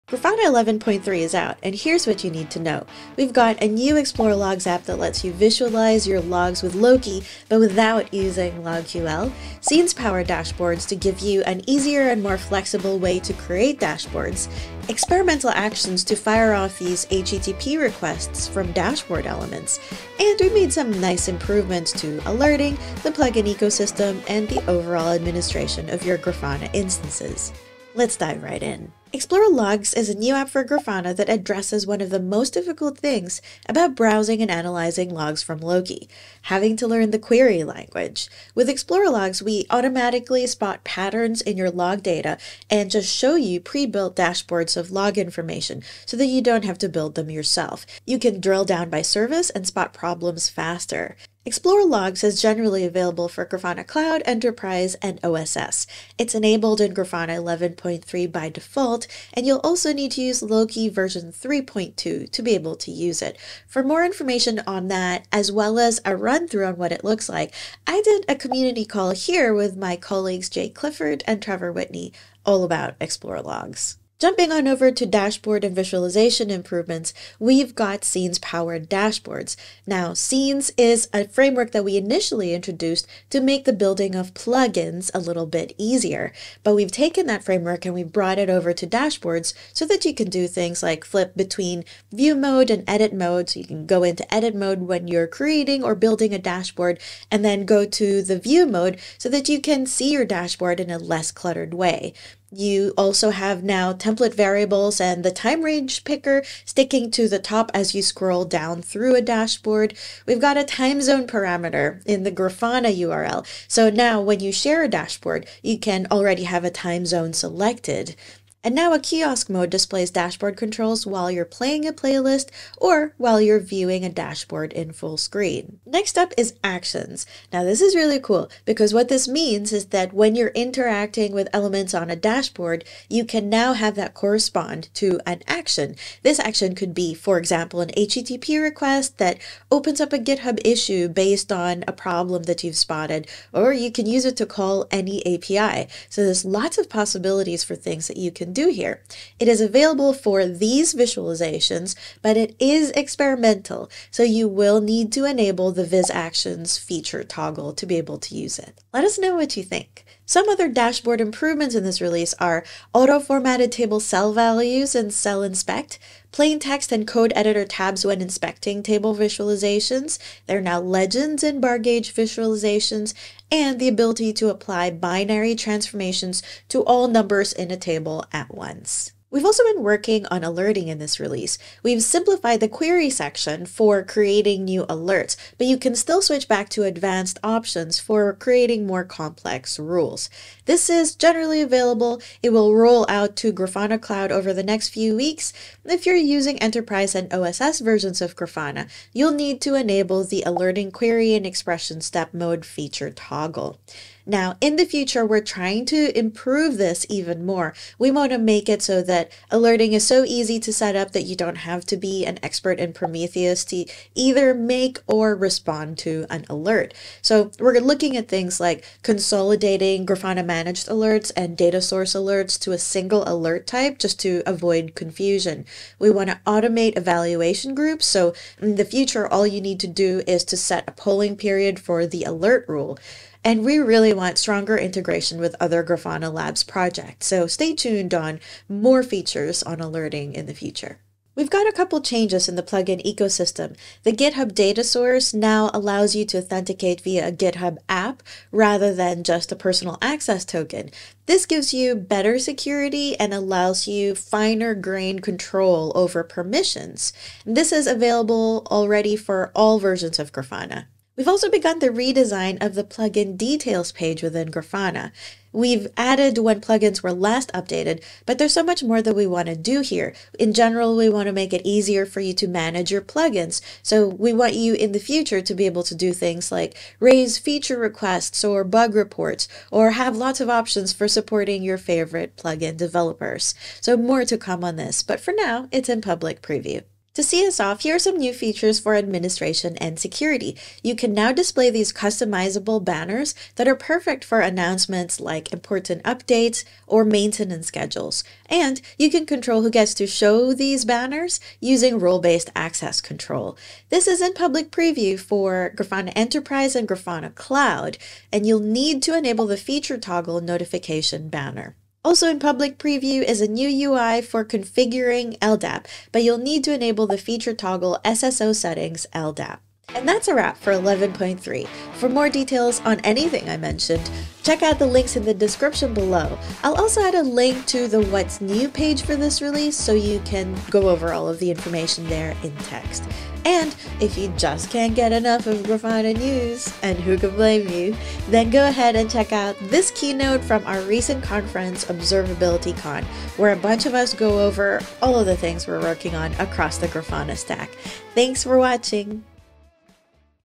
Grafana 11.3 Now GA! Here's the TL;DR
Welcome to Grafana 11.3! Scenes-powered dashboards are now generally available and the Explore Logs plugin is now installed by default. The dashboard experience has also improved in other ways including the ability to trigger API calls from any canvas element with the new Actions option and an update to transformations so you can apply calculations to dynamic fields. We’ve also simplified the alert setup experience, added customizable announcement banners that admins can send to all users, and improved some default permissions.

Chapters:
00:00 TL;DR of Grafana 11.3
00:29 Explore Logs app for Grafana and Loki
01:14 Scenes-powered dashboards
02:06 Actions
02:40 Other dashboard improvements
03:00 Alerting
04:01 Plugin ecosystem changes
04:47 Administration and security features